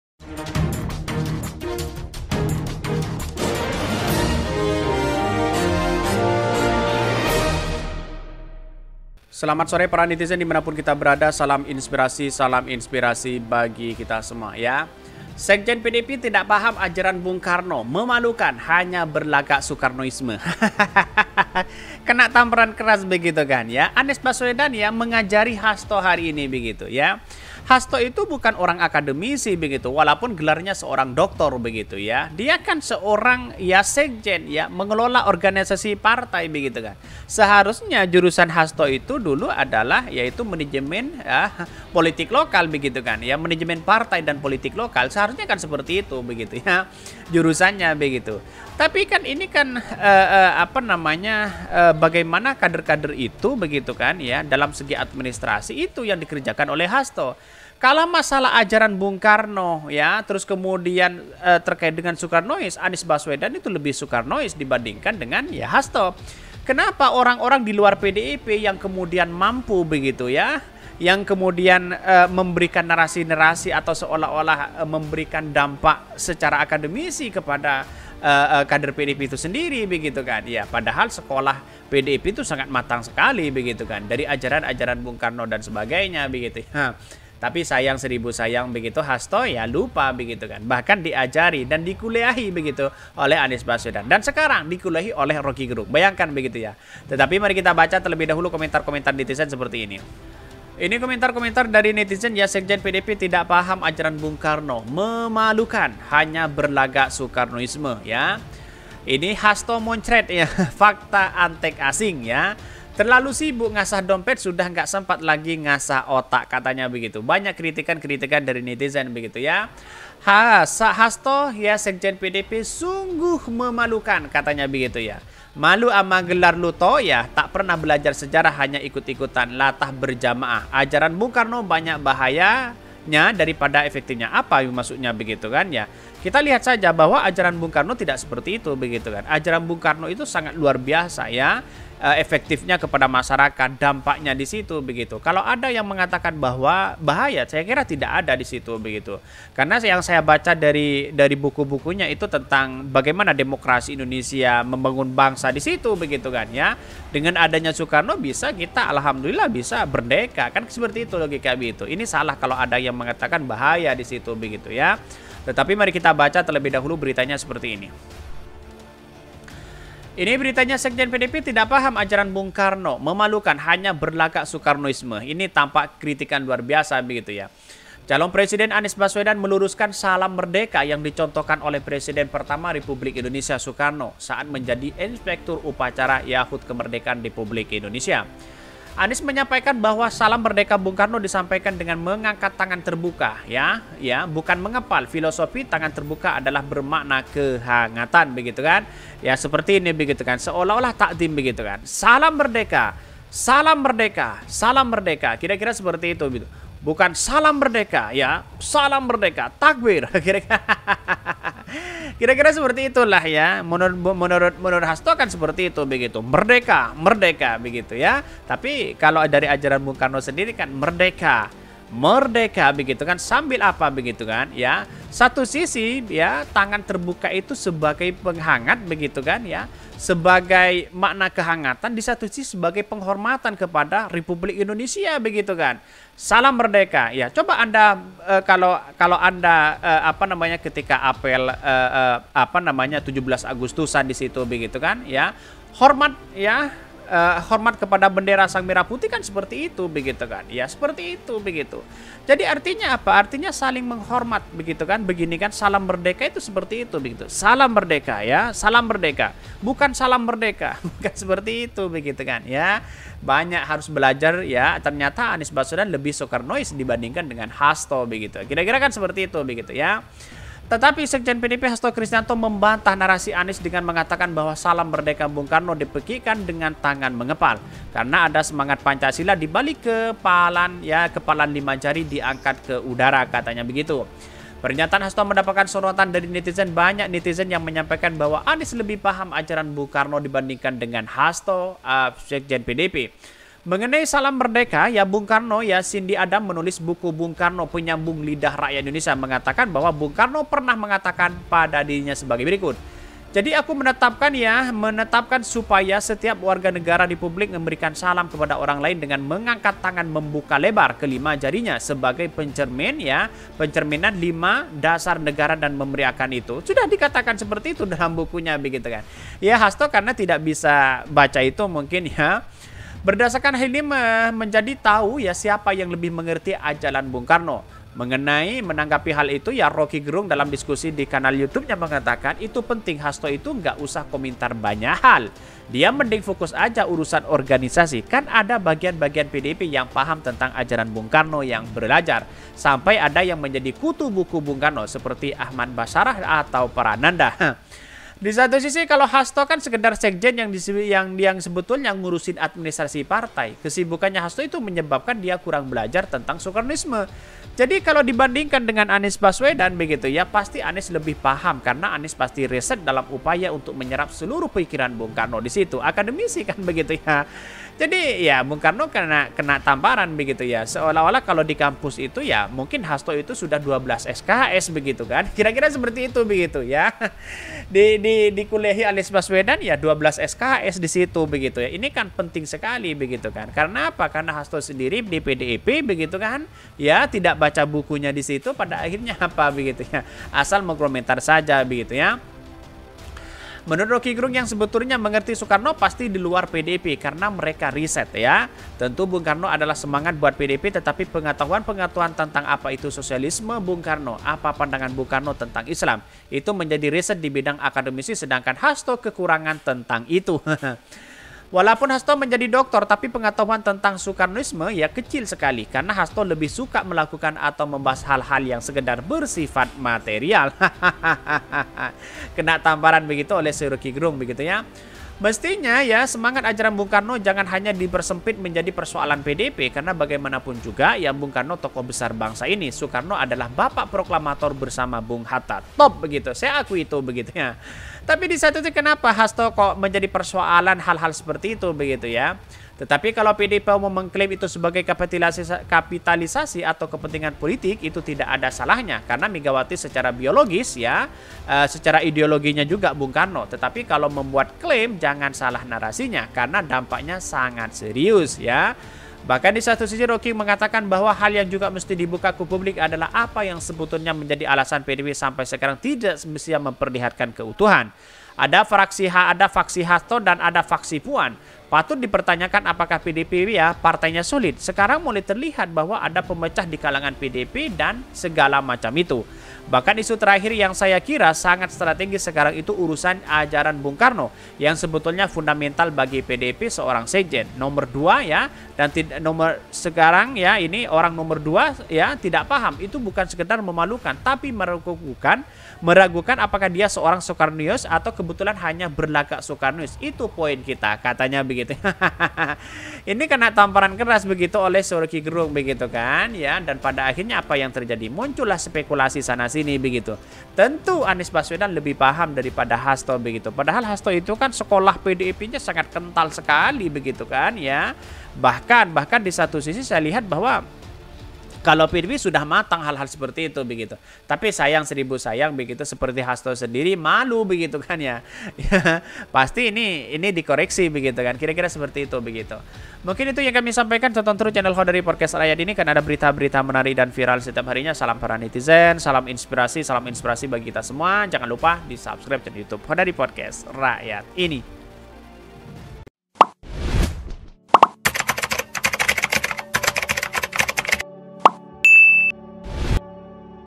MEMALUKAN...!! SEKJEN PDIP HASTO TIDAK PAHAM AJARAN BUNG KARNO; HANYA BERLAGAK SOEKARNOIS?
Calon presiden Anies Baswedan meluruskan salam merdeka yang dicontohkan oleh Presiden pertama Republik Indonesia, Soekarno, saat menjadi Inspektur Upacara pada HUT Kemerdekaan Republik Indonesia ke-78, Kamis (17/8/23) di Waduk Lebak Bulus, Jakarta Selatan.